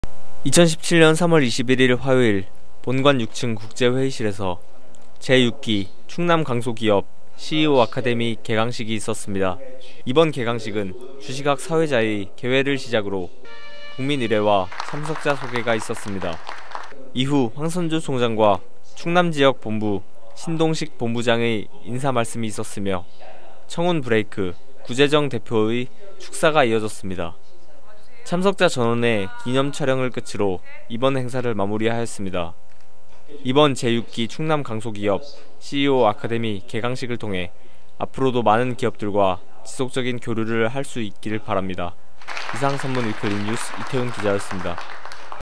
제6기 충남강소기업CEO아카데미 개강식
2017년 3월 21일 화요일 본관 6층 국제회의실에서 제6기 충남강소기업 CEO아카데미 개강식이 있었습니다.

이번 개강식은 주시각 사회자의 개회를 시작으로 국민의례와 참석자 소개가 있었습니다. 이후 황선조 총장과 충남 지역본부 신동식 본부장의 인사말씀이 있었으며, 청운 브레이크 구제정 대표의 축사가 이어졌습니다. 참석자 전원의 기념촬영을 끝으로 이번 행사를 마무리하였습니다.

이번 제6기 충남강소기업CEO 아카데미 개강식을 통해 앞으로도 많은 기업들과 지속적인 교류를 할 수 있길 바랍니다.

이상 Sun Moon Weekly News 이태훈기자 였습니다.